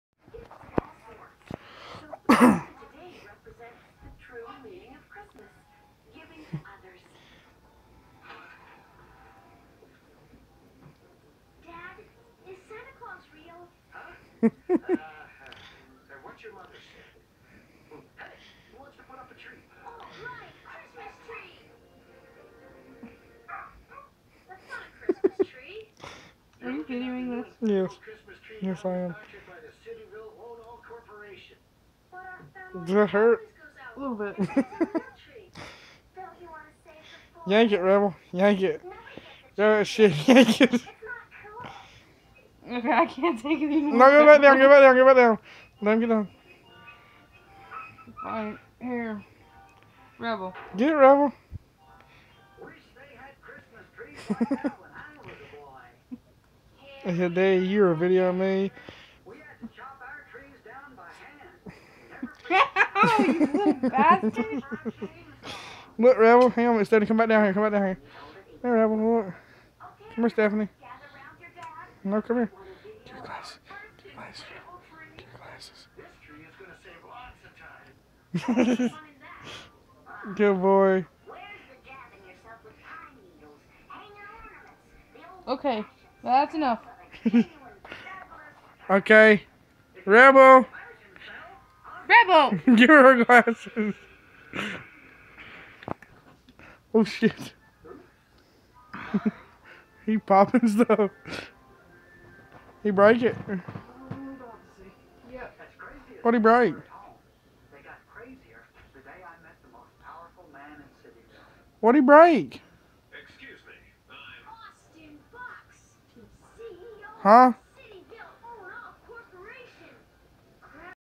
rebel the pitbull vs glasses
Rebel's antics continue with eating my wife's hair and glasses!